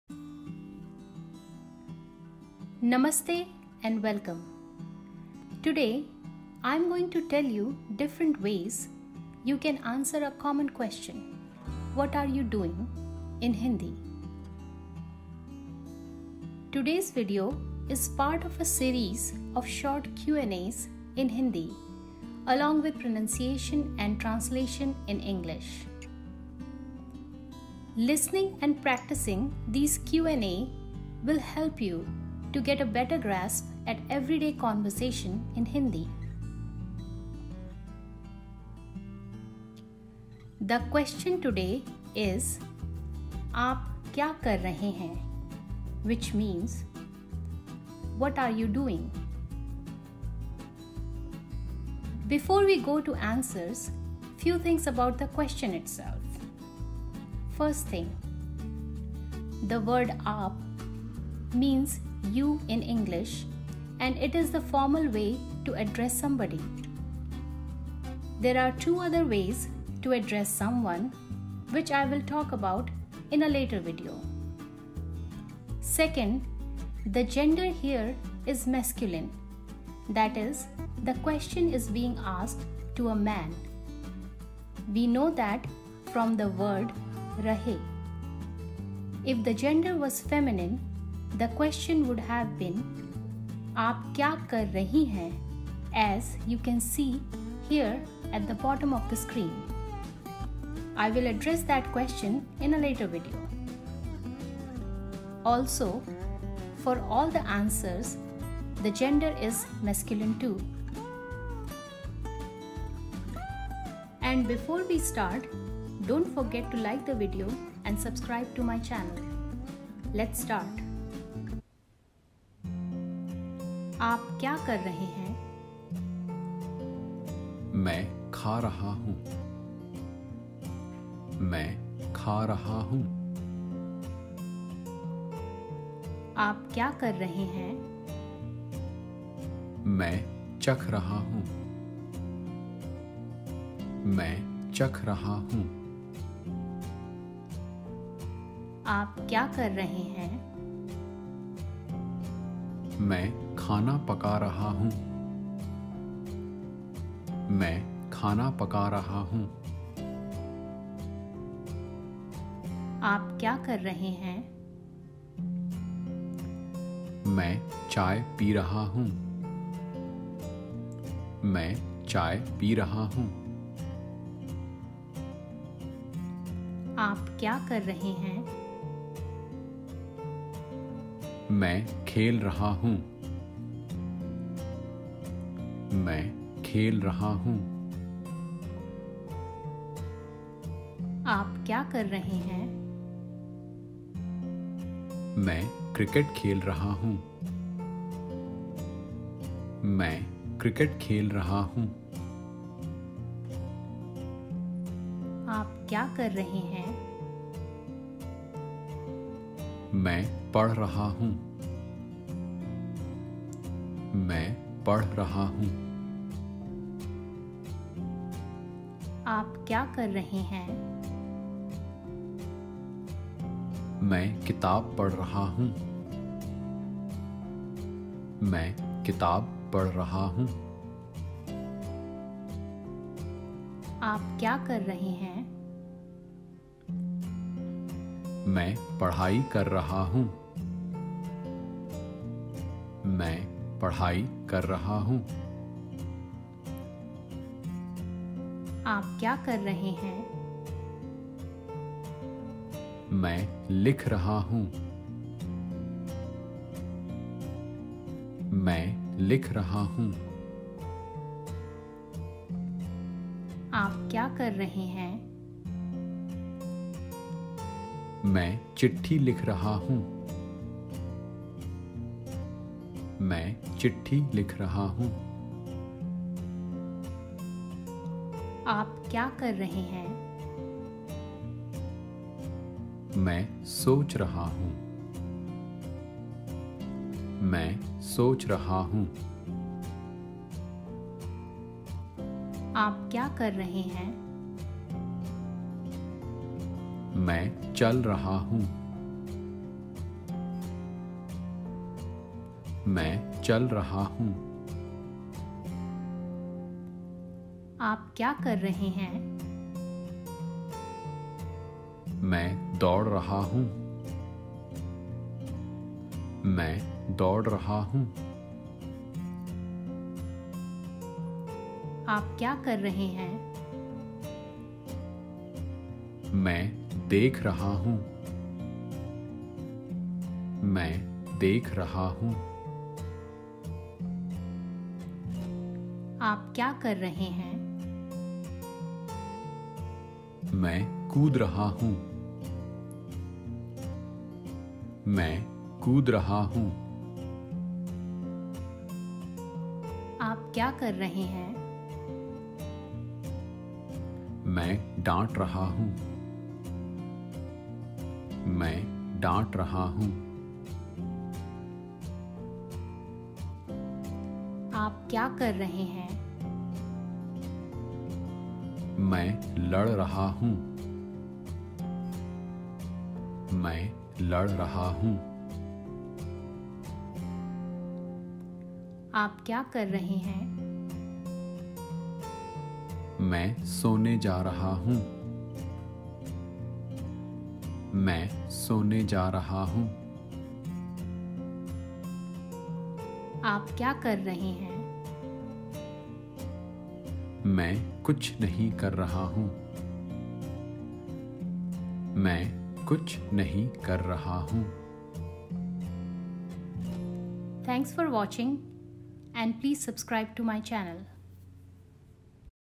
Q&A in Hindi for Beginners with pronunciation and translation in English #2
Namaste and welcome. Today I am going to tell you different ways you can answer a very common question “What are you doing?” in Hindi.
Today’s video is a part of a series of short Q &As in Hindi along with pronunciation and translation in English.  Listening and practicing these Q and A will help you get a better grasp at everyday conversation in Hindi.
The question today is “Aap kyaa kar rahe hain” or What are you doing?